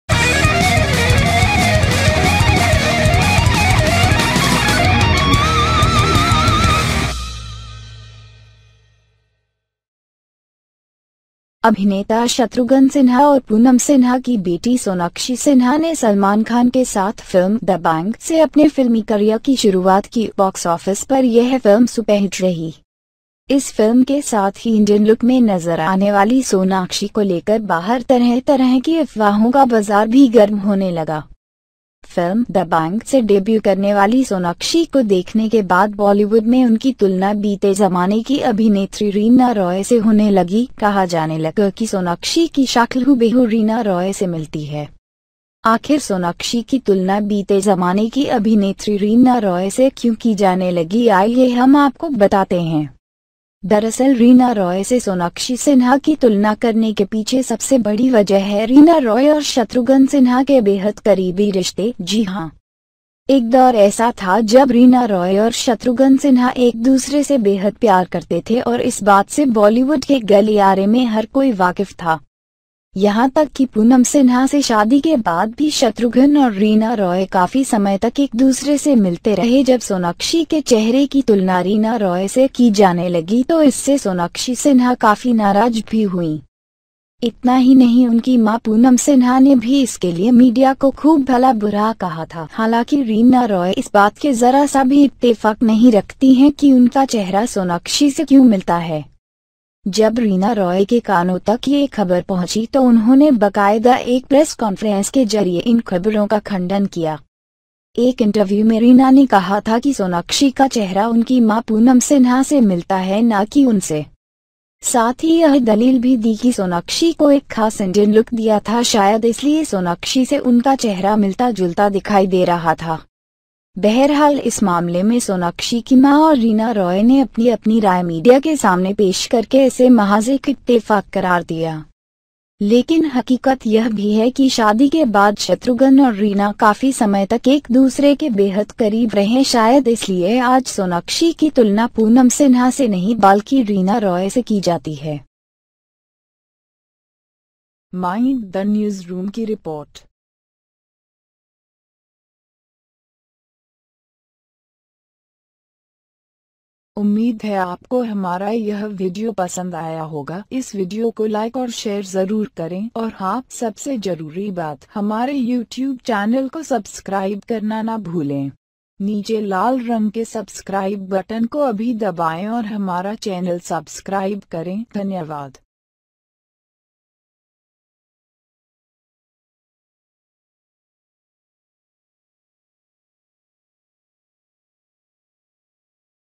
Sonakshi Sinha, Shatrughan Sinha और Reena Roy की अवैध संतान है? खुद देखें और बताएं यह सच है या झूठ.
सोनाक्षी सिन्हा, शत्रुघ्न सिन्हा और रीना रॉय की अवैध संतान है? खुद ही देख लीजिये और बताएं की यह सच है या झूठ.
It is said that Bollywood actress Sonakshi Sinha is an illegitimate child of Shatrughan Sinha and Reena Roy? Watch this video and find it for yourself and tell us if it is true by commenting below.
अभिनेता शत्रुघ्न सिन्हा और पूनम सिन्हा की बेटी सोनाक्षी सिन्हा ने सलमान खान के साथ फिल्म 'दबंग' से अपने फिल्मी करियर की शुरूआत की. बॉक्स ऑफिस पर यह फिल्म सुपरहिट रही. इस फिल्म के साथ ही इंडियन लुक में नजर आनेवाली सोनाक्षी को लेकर बाहर तरह-तरह की अफवाहों का बाजार भी गर्म होने लगा. फिल्म 'दबंग' से डेब्यू करनेवाली सोनाक्षी को देखने के बाद बॉलीवुड में उनकी तुलना बीते जमाने की अभिनेत्री रीना रॉय से होने लगी. कहा जाने लगा कि सोनाक्षी की शक्ल हुबहु रीना रॉय से मिलती है.